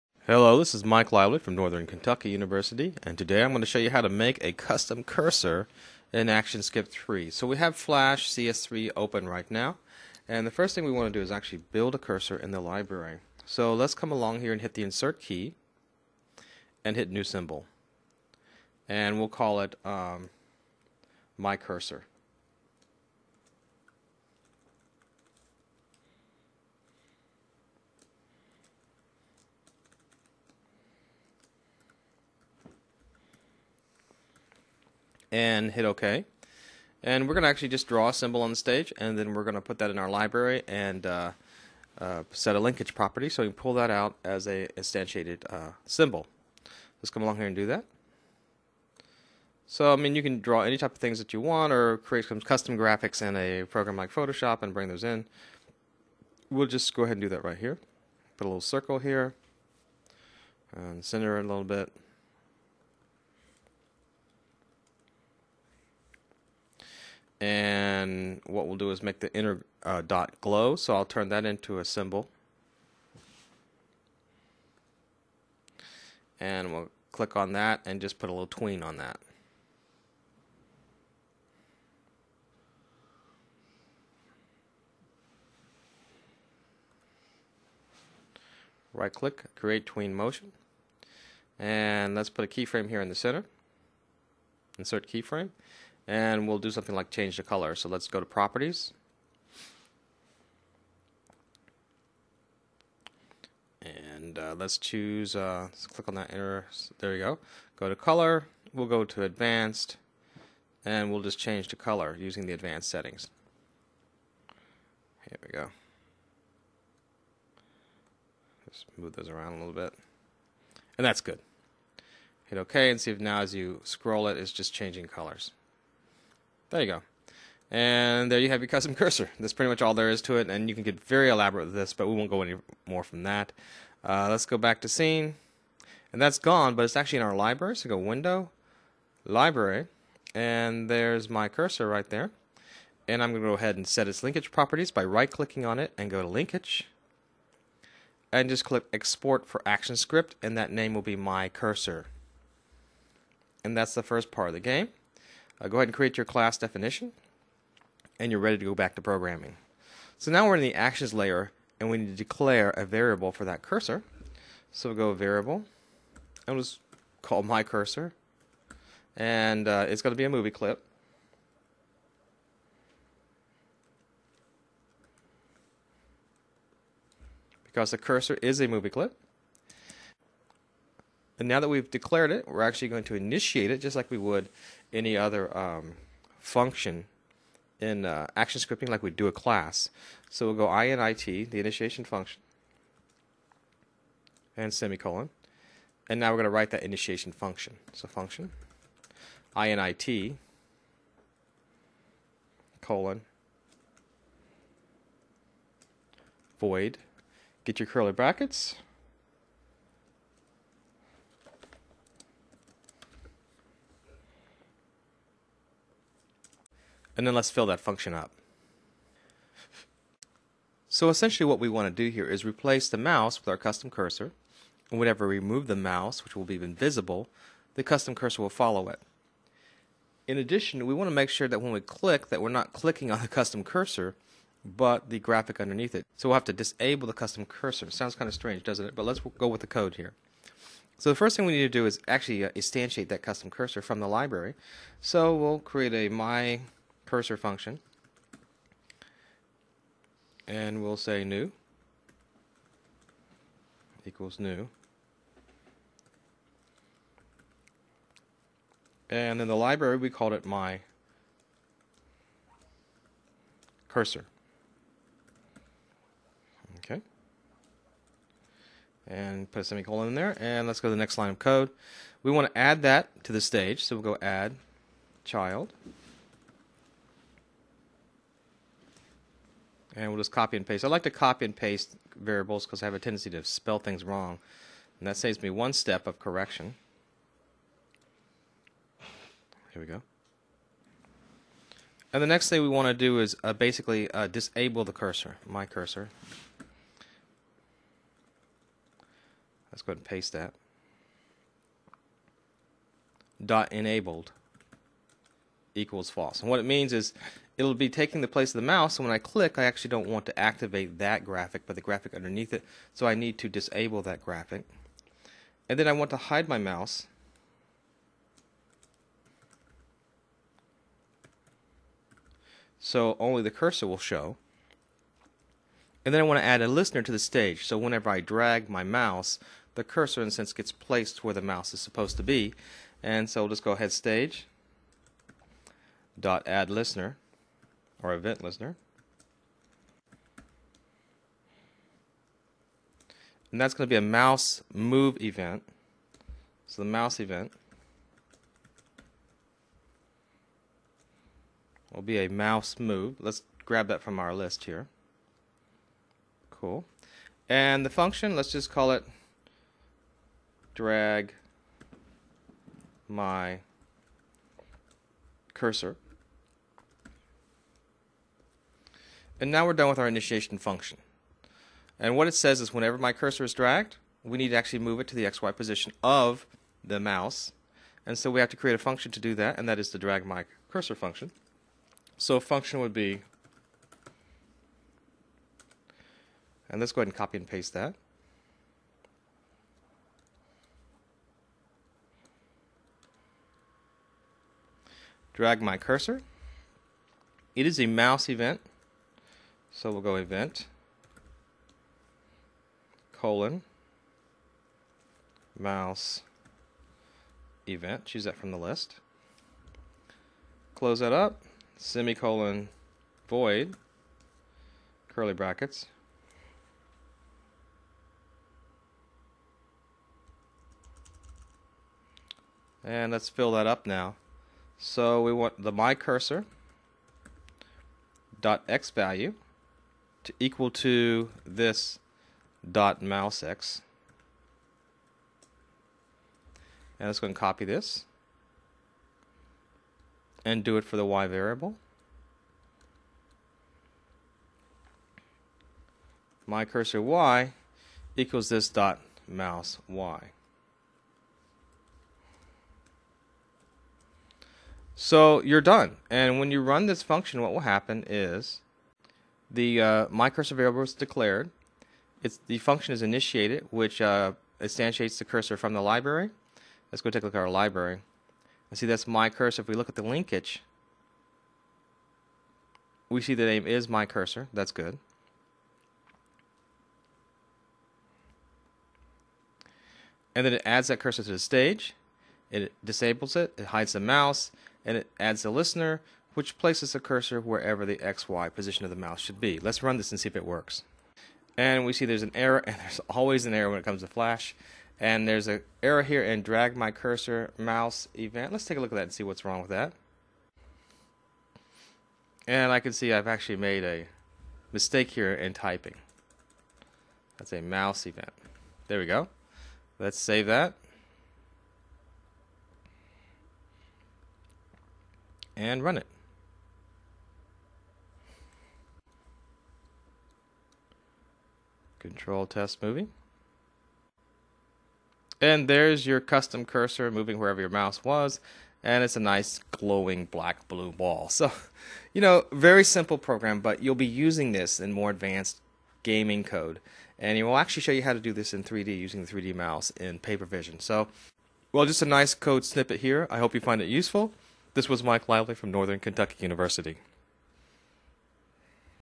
Creating a Custom Game Cursor in AS3
Tutorial illustrates how to create a custom cursor in AS3 for Flash
game programming.

This tutorial was created by Mike Lively of NKU.